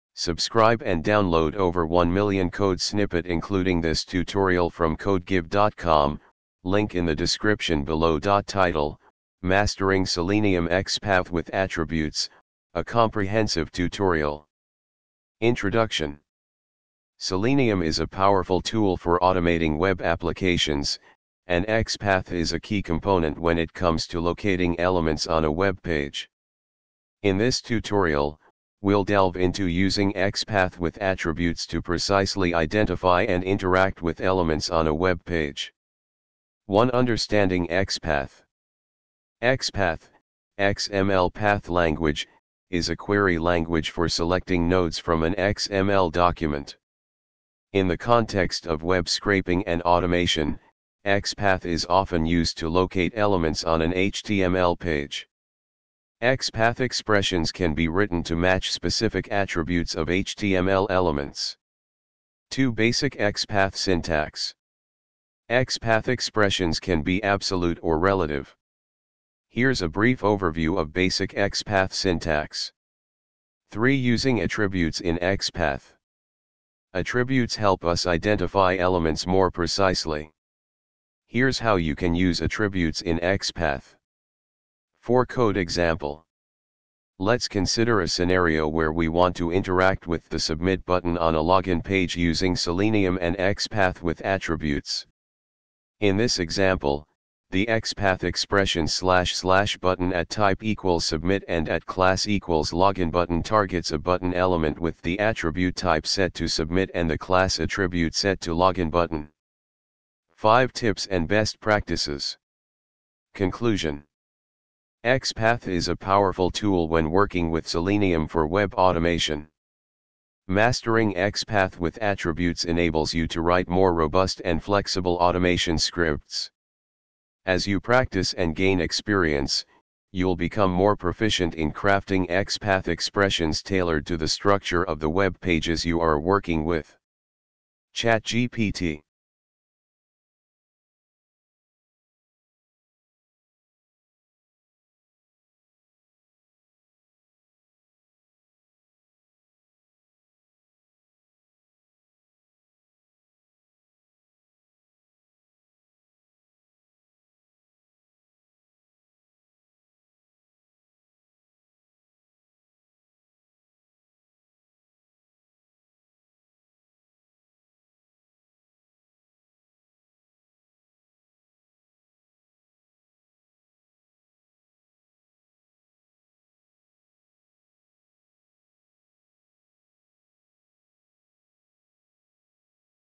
selenium xpath with attribute
Title: Mastering Selenium XPath with Attributes - A Comprehensive Tutorial
Introduction:
Selenium is a powerful tool for automating web applications, and XPath is a key component when it comes to locating elements on a web page. In this tutorial, we'll delve into using XPath with attributes to precisely identify and interact with elements on a webpage.
1. Understanding XPath:
XPath (XML Path Language) is a query language for selecting nodes from an XML document. In the context of web scraping and automation, XPath is often used to locate elements on an HTML page. XPath expressions can be written to match specific attributes of HTML elements.
2. Basic XPath Syntax:
XPath expressions can be absolute or relative. Here's a brief overview of basic XPath syntax:
3. Using Attributes in XPath:
Attributes help us identify elements more precisely. Here's how you can use attributes in XPath:
4. Code Example:
Let's consider a scenario where we want to interact with the "Submit" button on a login page using Selenium and XPath with attributes.
In this example, the XPath expression //button[@type='submit' and @class='login-button'] targets a button element with the attribute type set to 'submit' and the class attribute set to 'login-button'.
5. Tips and Best Practices:
Conclusion:
XPath is a powerful tool when working with Selenium for web automation. Mastering XPath with attributes enables you to write more robust and flexible automation scripts. As you practice and gain experience, you'll become more proficient in crafting XPath expressions tailored to the structure of the web pages you are working with.
ChatGPT